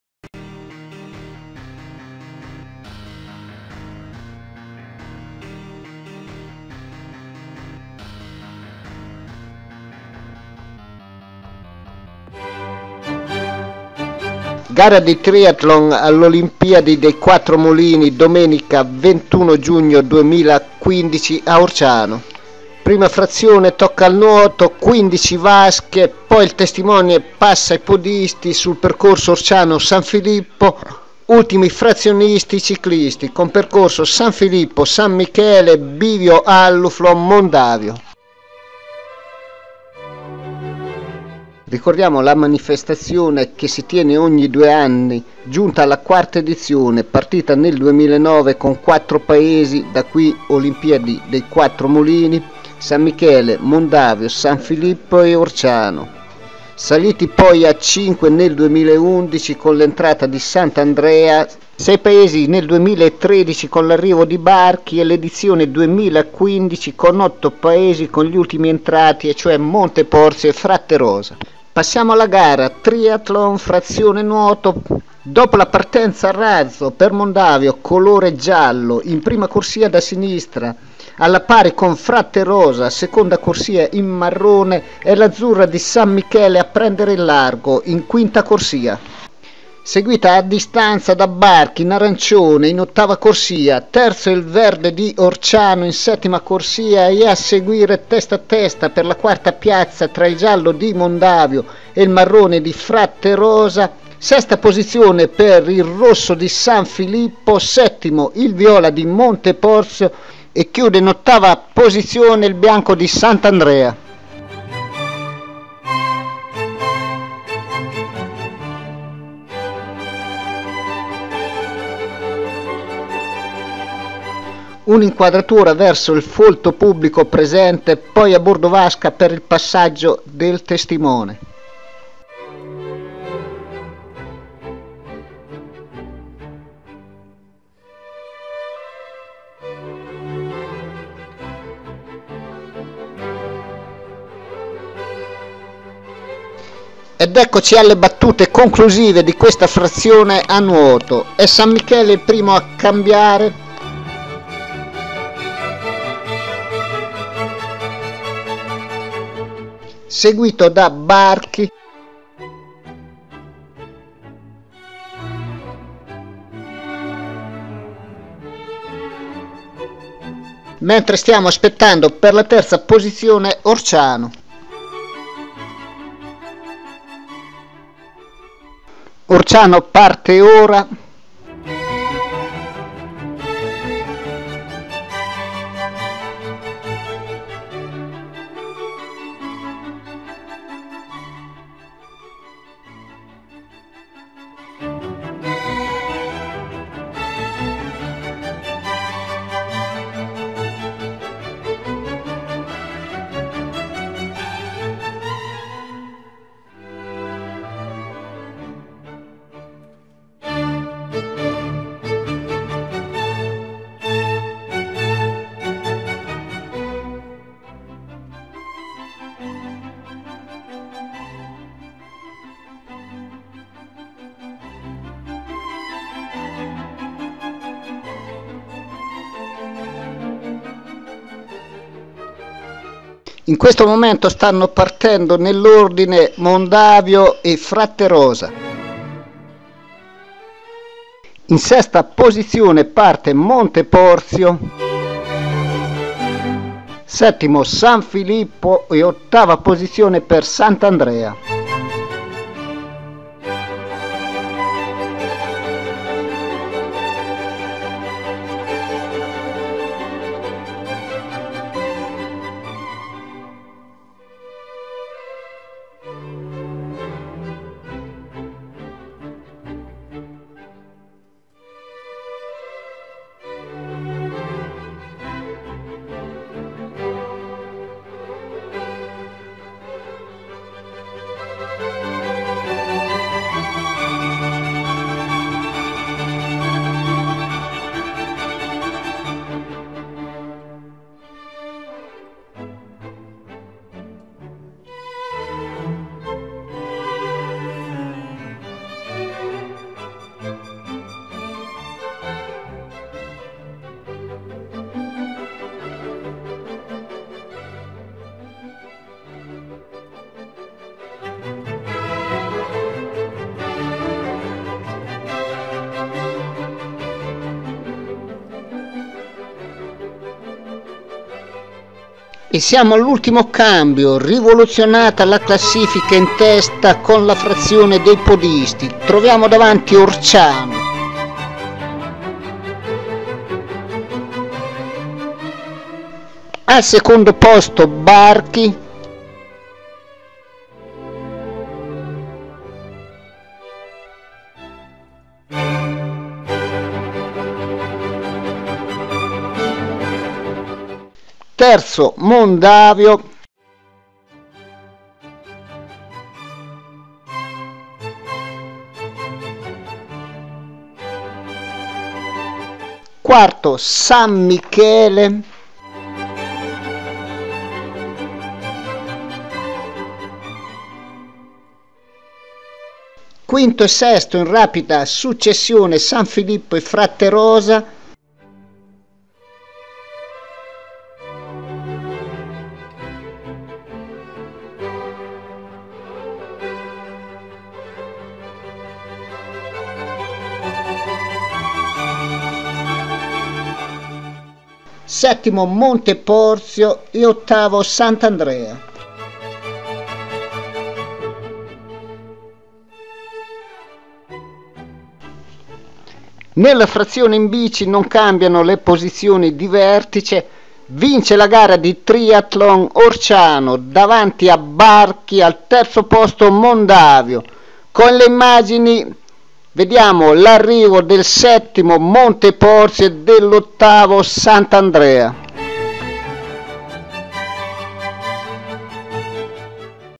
Olimpiadi dei 4 Mulini: Triathlon by Teleskianto
Una delle tante gare di queste Olimpiadi dei 4 Mulini giunta alla 4° edizione